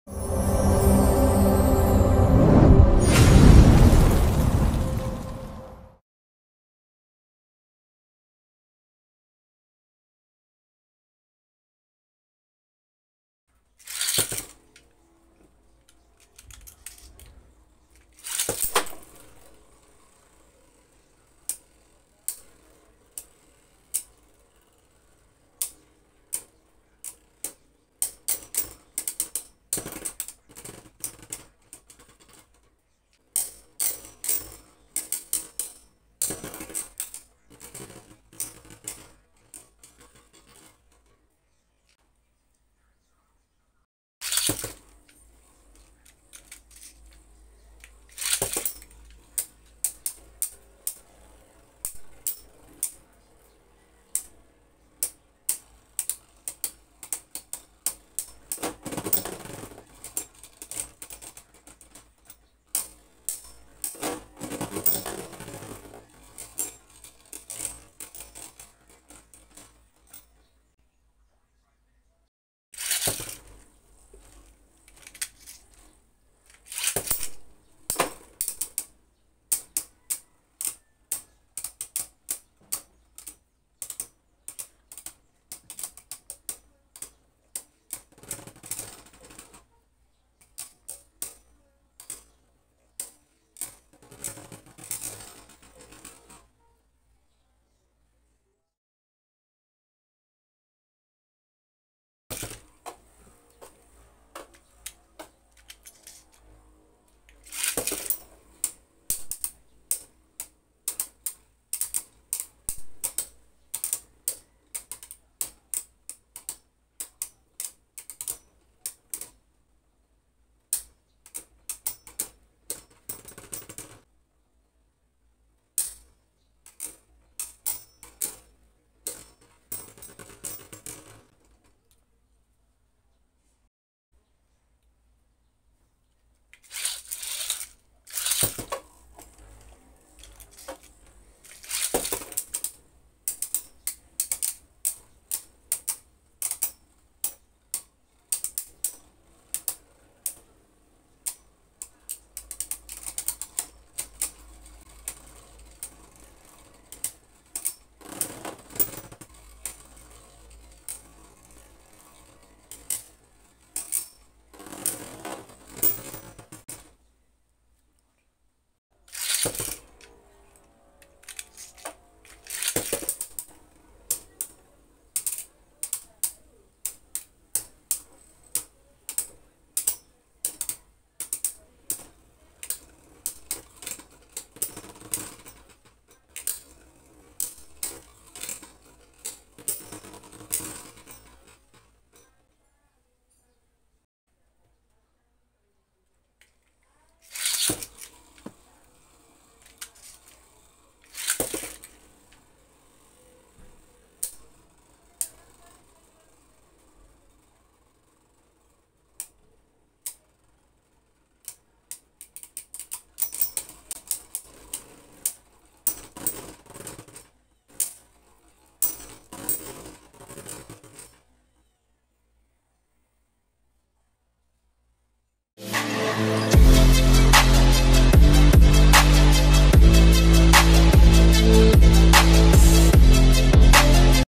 Infinite Achilles Sword/Shield vs Dynamite Belial || Beyblade Burst DB
Hello Bladers! Today we have a new exciting battle between Infinite Achilles and Dynamite Belial! This battle would be exciting! Who do you think will win?

Make sure to Like and Follow our Facebook Page for more!